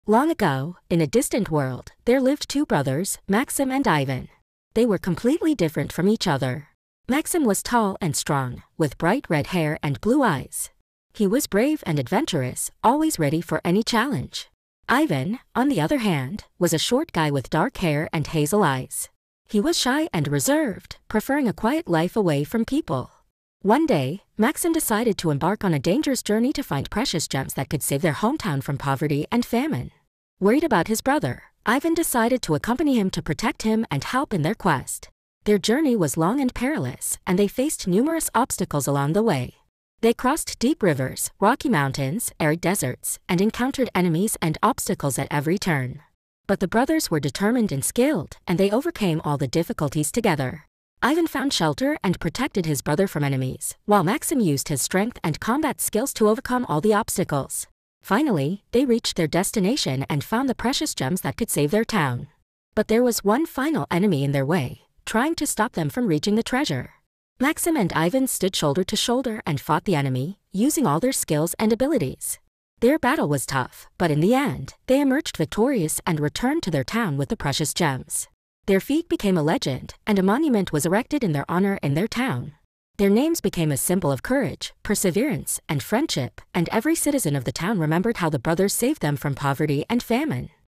"Brothers on the Path to Treasures"
Description: A story about two brothers, Maxim and Ivan, who set out on a dangerous journey to save their city from poverty and hunger. They overcame all the difficulties together, defeated enemies and found precious stones. Their feat became a legend and a symbol of courage and perseverance.